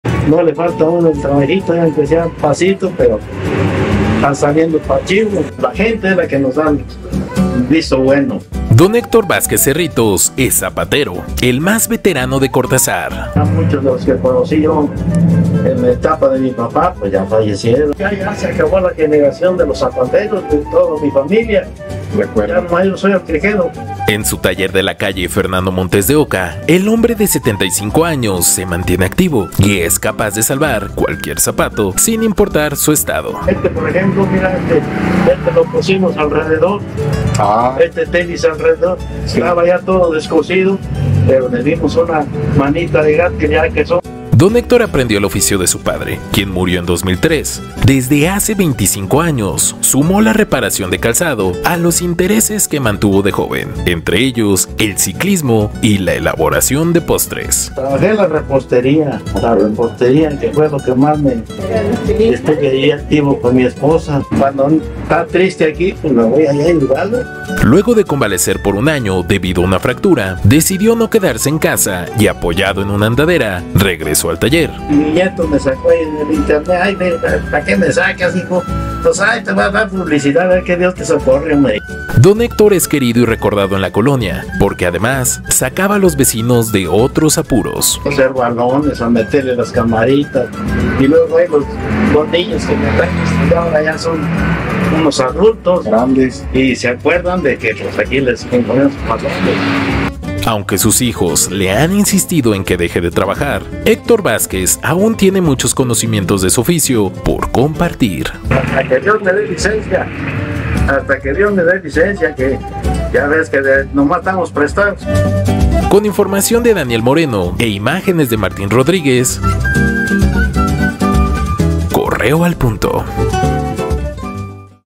Don Héctor ha sido el reparador de zapatos de Cortazar desde hace 25 años
El reparador de zapatos de Cortazar ha dedicado su vida a esta labor y no piensa abandonar su trabajo mientras pueda continuar haciéndolo